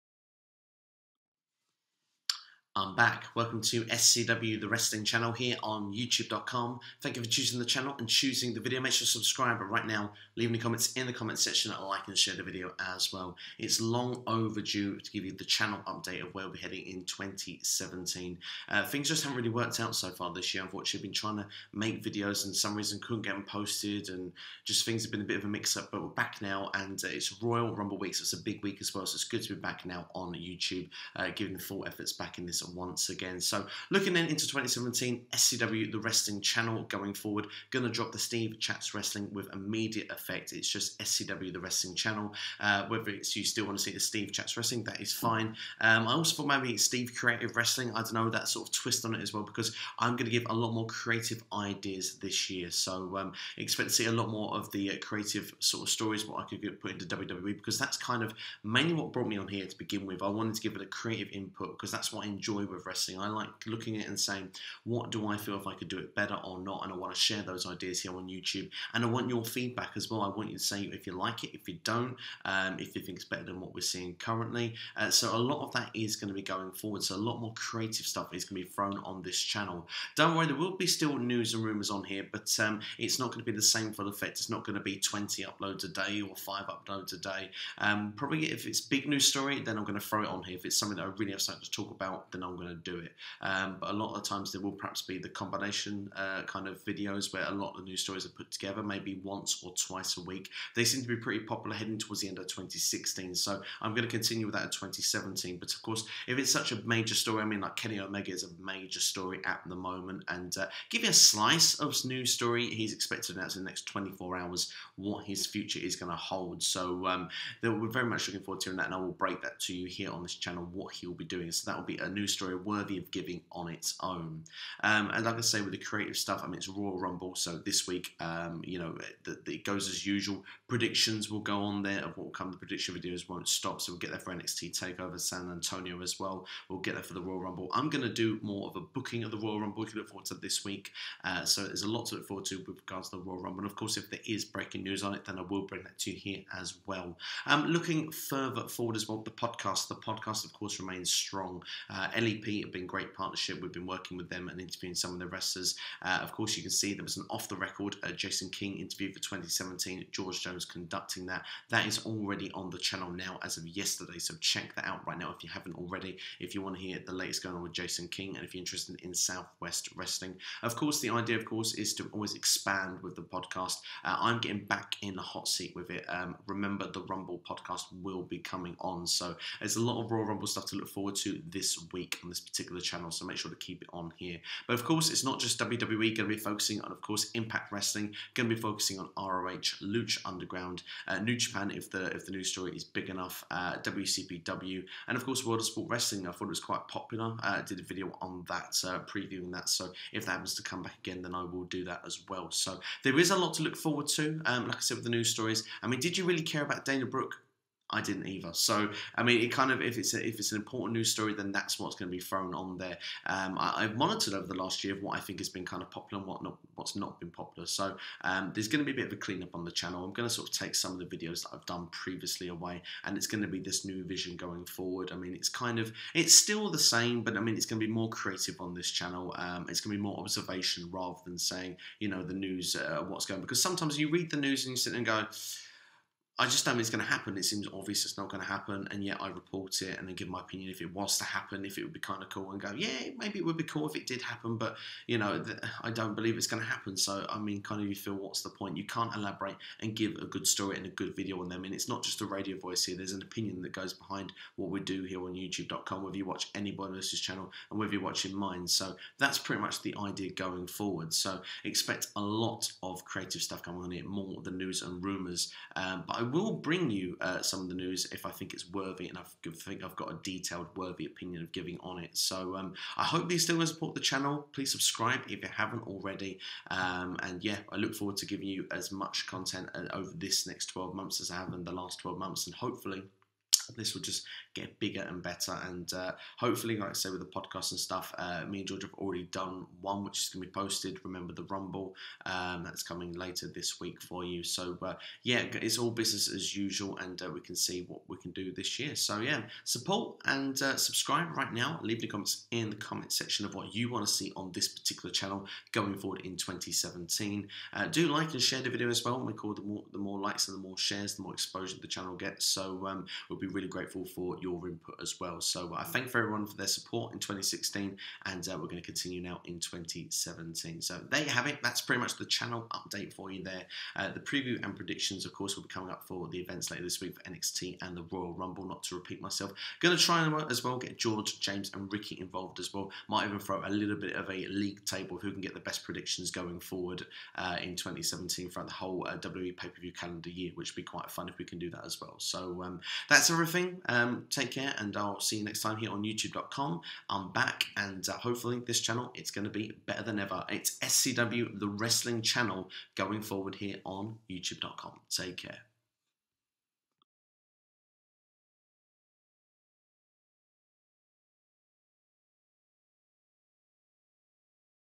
SCW The Wrestling Channel Update! Everything You Need To Know About This Channel!
SCW The Wrestling Channel Update!

Giving you all the updates regarding this channel that you need to know.  Let me know your feelings in the comments below.